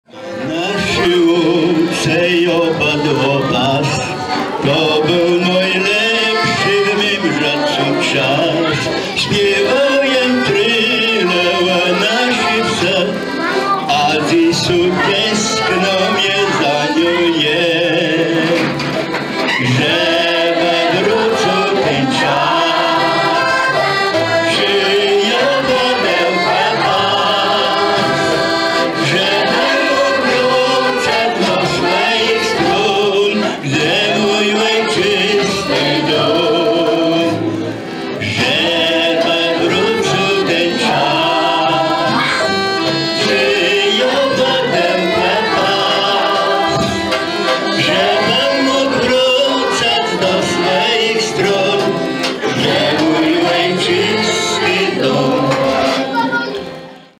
coolio śpiewa po kaszubsku
archiwalne nagranie z biesiady teatralnej